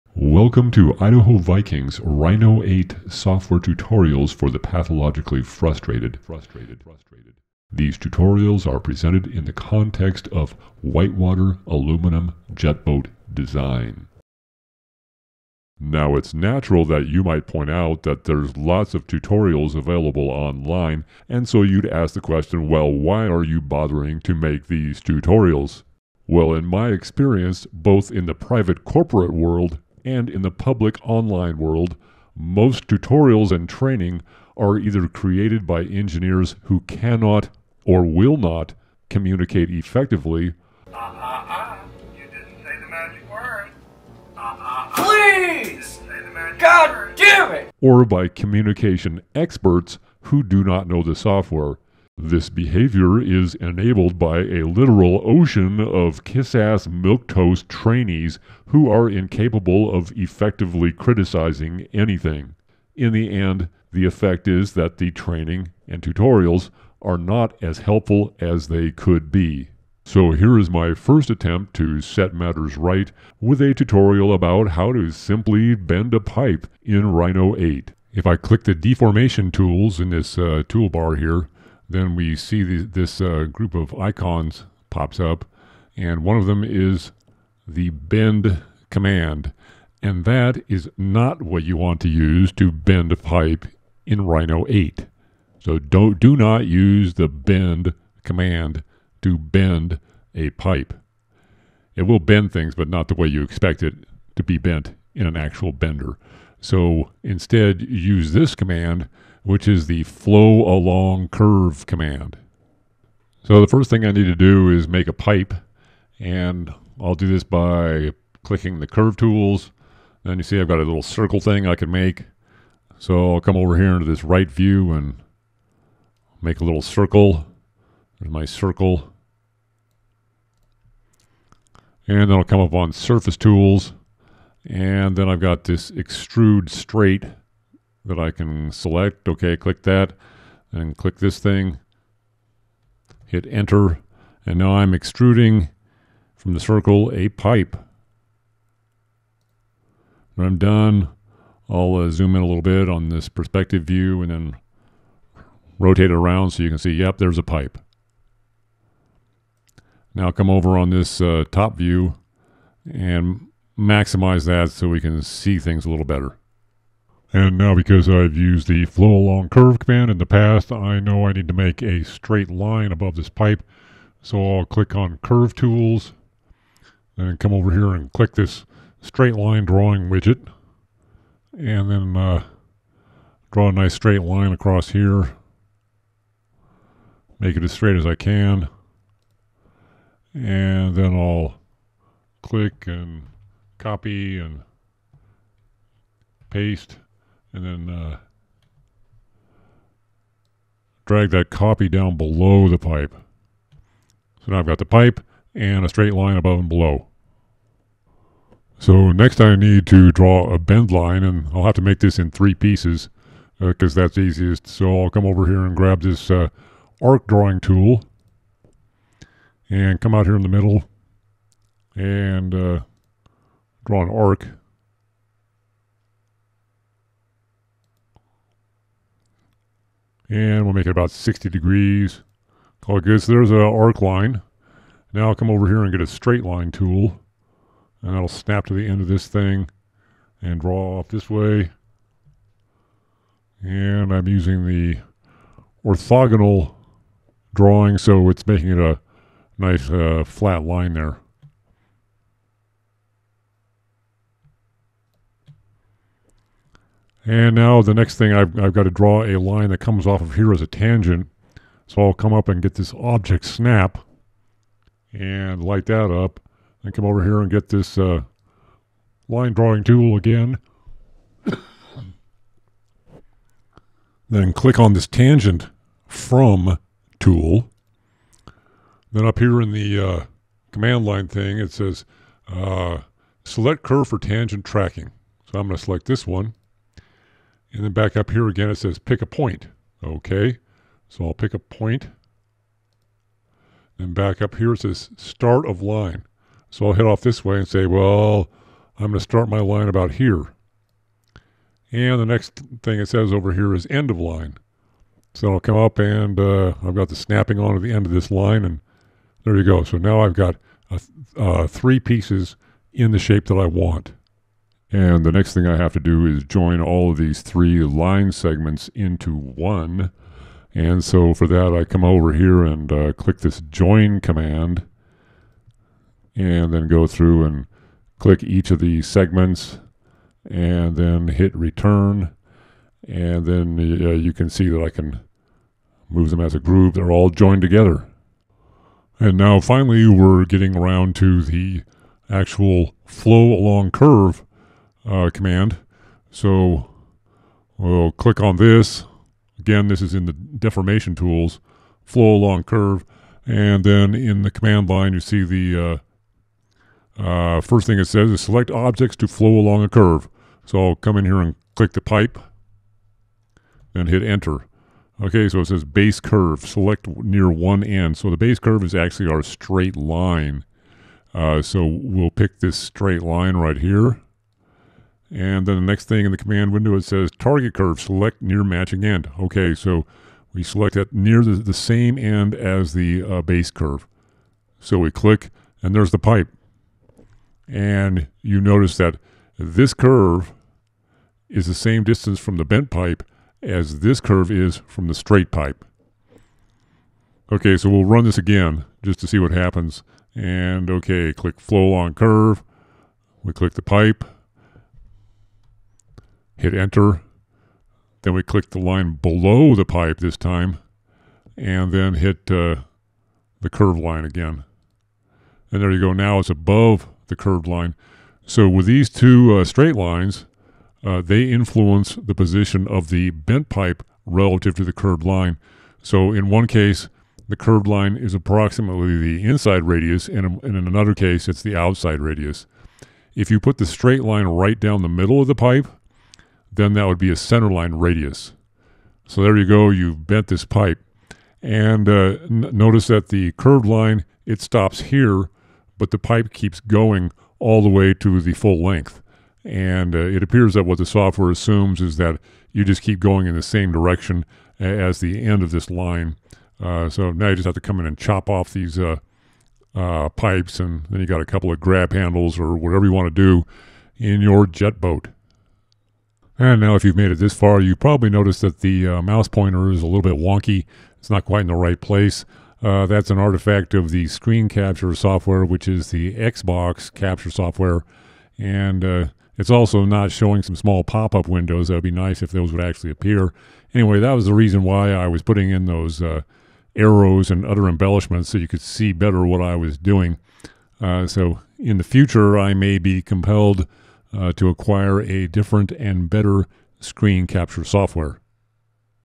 Rhino 8 Bend a Pipe Tutorial
Bend a pipe tutorial for Rhino 8. It's not obvious how to proceed, but once the method is clear, then it is easy.

This is the 3rd upload of this video, with small revisions, because my quality control department had its head in the brown place.